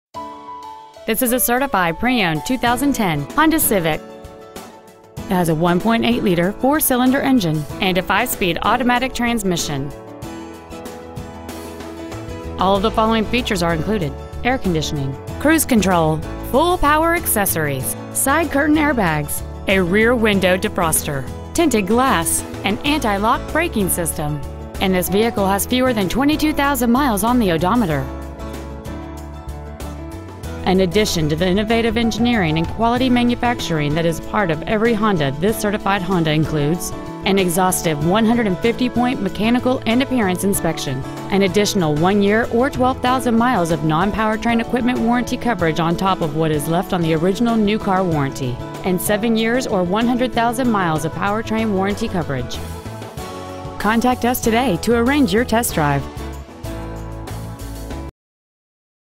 Preowned 2010 Honda Civic Madison WI
We have been honored to serve the Madison WI area , we promise that your experience at our dealership will exceed your expectations ! Year : 2010 Make : Honda Model : Civic Engine : 1.8L 4 cyls Trans . : Automatic 5-Speed Exterior : Silver Miles : 21,642 Stock : 10998A Wilde East Towne Honda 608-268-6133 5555 High Crossing Blvd.